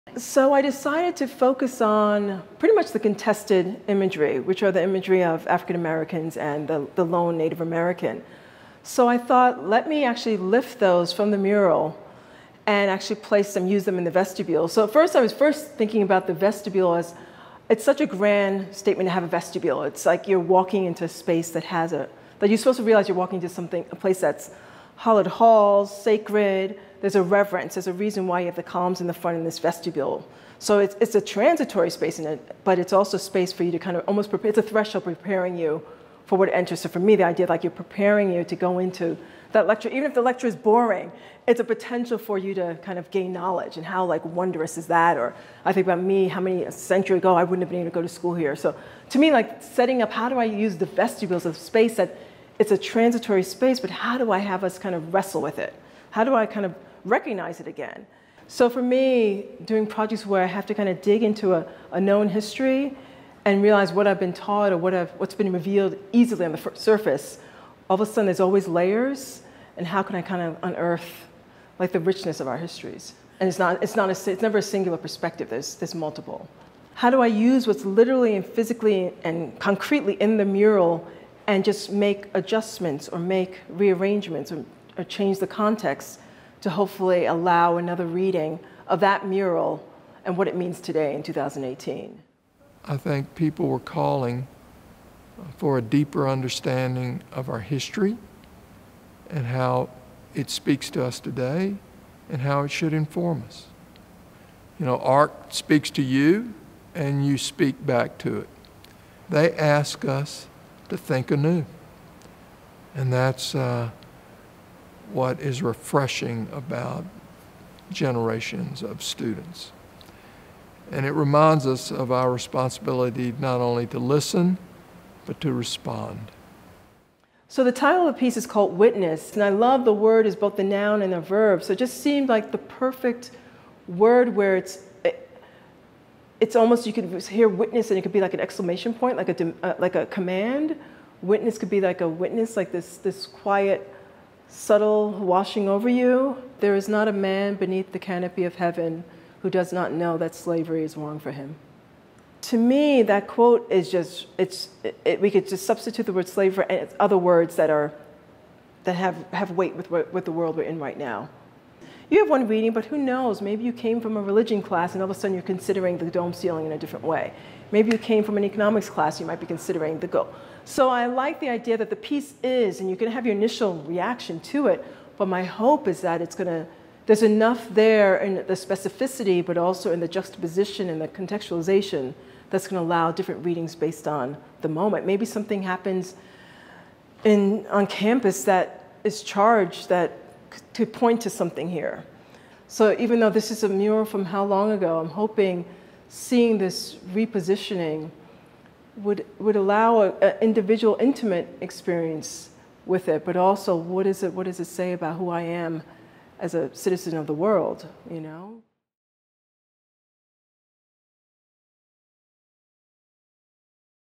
Memorial Hall Visitors ‘Witness’ New Perspective on Kentucky History
Artist Karyn Olivier's gold-leafed artwork, which features African-American and Native American images, hopes to shine new light on many misrepresented Kentuckians from the state’s history.